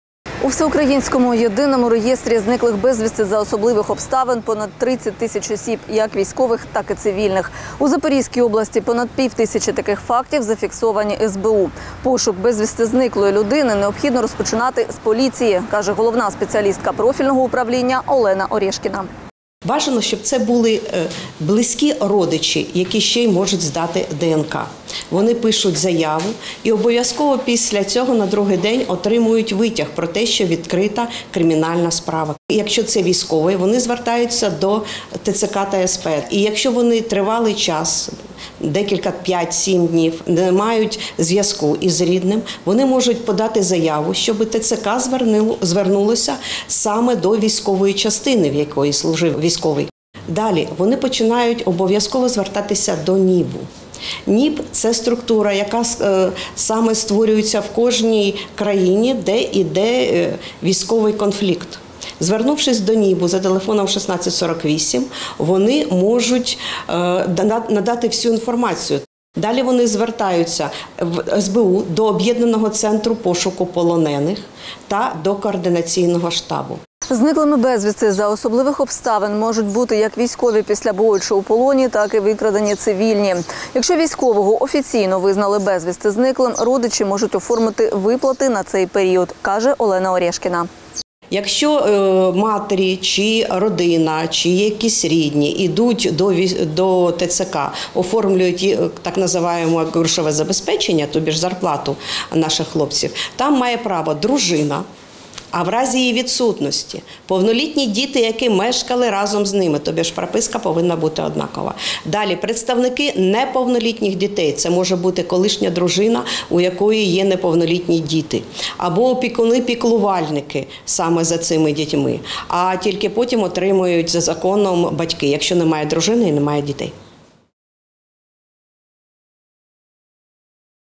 У Запорізькій області зафіксовано понад 500 випадків зникнення безвісти цивільних людей
У всеукраїнському Єдиному реєстрі зниклих безвісти за особливих обставин понад 30 тисяч людей як військових, так і цивільних. У Запорізькій області - понад пів тисячі таких фактів зафіксовані СБУ. Пошук безвісти зниклої людини необхідно розпочинати з поліції, каже головна спеціалістка профільного управління Олена Орєшкіна.